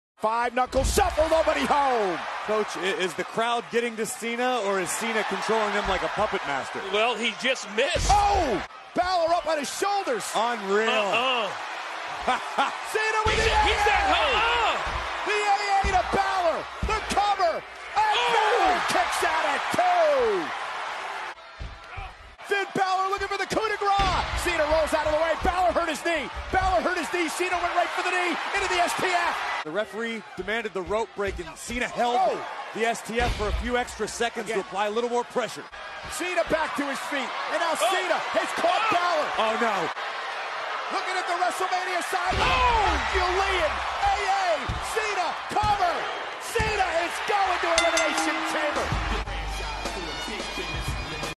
WWE John Cena: Defeat Prince Devitt With Two EFU
Here In This Video Let's See, Like John Cena: defeat Prince Devitt with two EFU